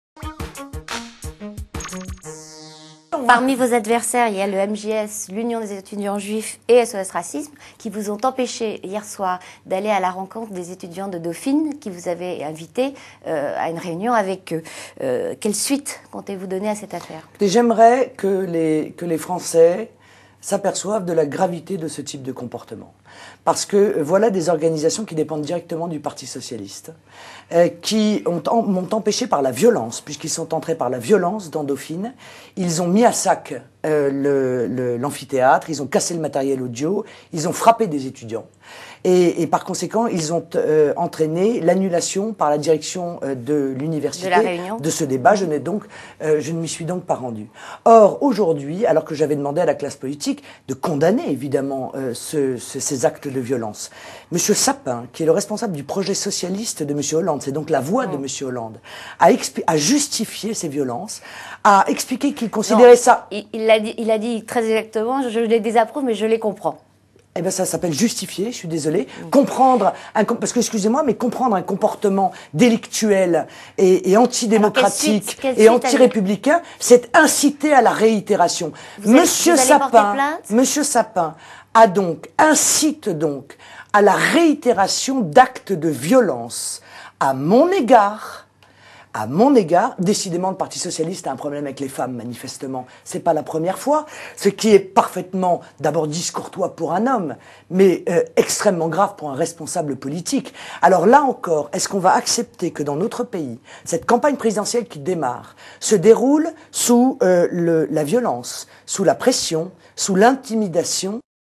Marine Le Pen
"L'immense majorité des électeurs de l'UMP se sent beaucoup plus proche de notre projet que de la politique menée par Nicolas Sarkozy".